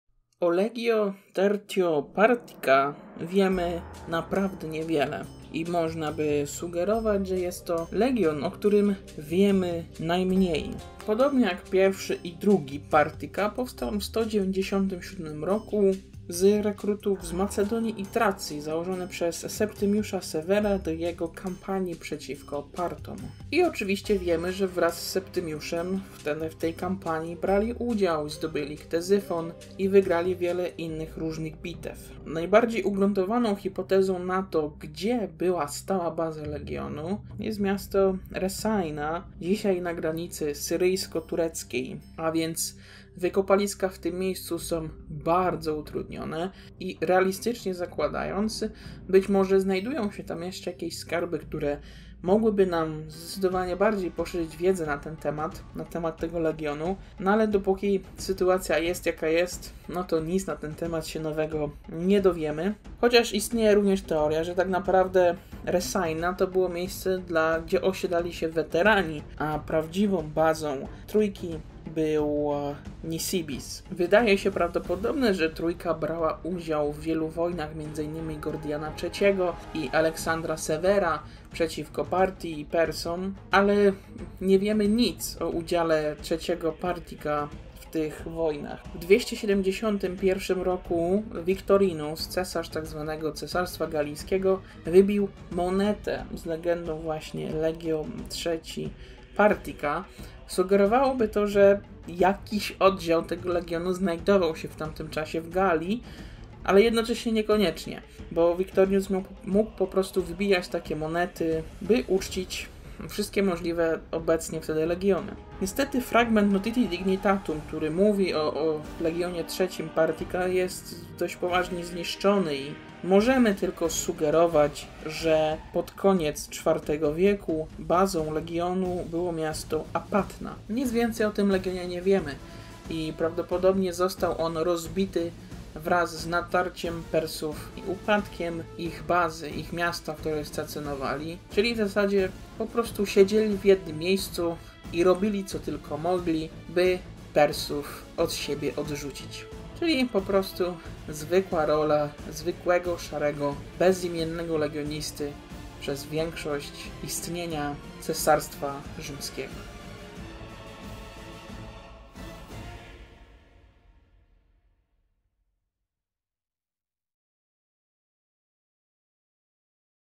Legio III Parthica - Legion widmo | Historia Legionów Rzymskich Odc. 15
O tym legionie w zasadzie nie wiemy nic, no może poza tym, kto i kiedy go założył. Zaskakująca jest ta cisza w źródłach. Jakaś taka niepokojąca. Melancholijna.

Źródła:
Dando-Collins, S. - Legions of Rome
Pollard, N. & Berry, J. - The Complete Roman Legions